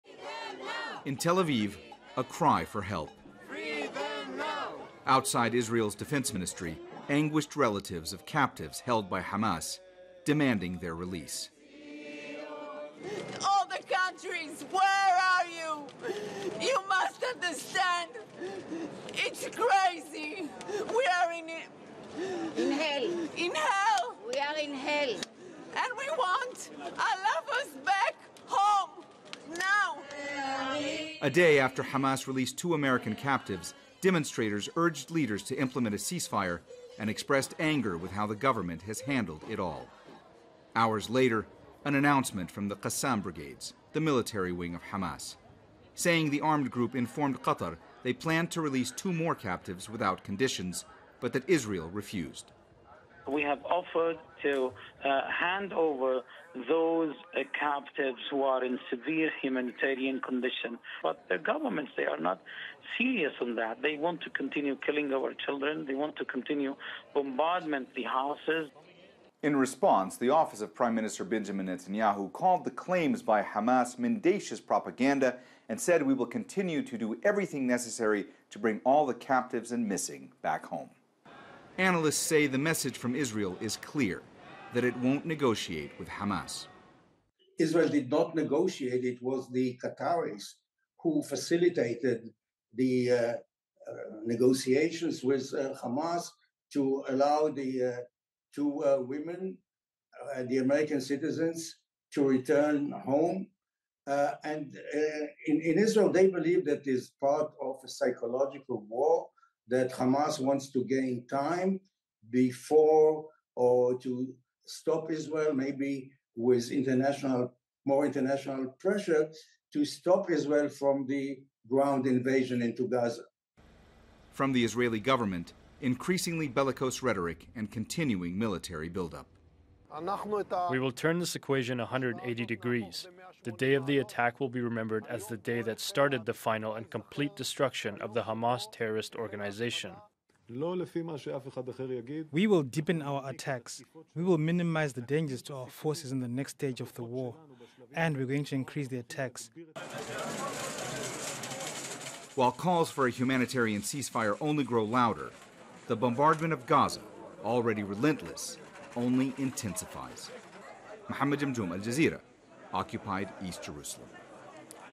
Hamas says Israel refused to receive 2 hostages; Israel calls it propaganda
Hamas says it offered to release two Israelis captured during its deadly raid but Israel’s government refused to take them. Israel described the claim as “mendacious propaganda”.
Abu Obeida, a spokesman for the al-Qassam Brigades, Hamas’s armed wing, said that the mediator Qatar was told of the group’s intention to release the Israelis on Friday, the same day it freed Americans Judith Tai Ranaan and her daughter Natalie.
In Tel Aviv, crowds of Israelis rallied demanding more be done to secure the release of those held in Gaza.

Al Jazeera’s Mohammed Jamjoom reports from occupied East Jerusalem.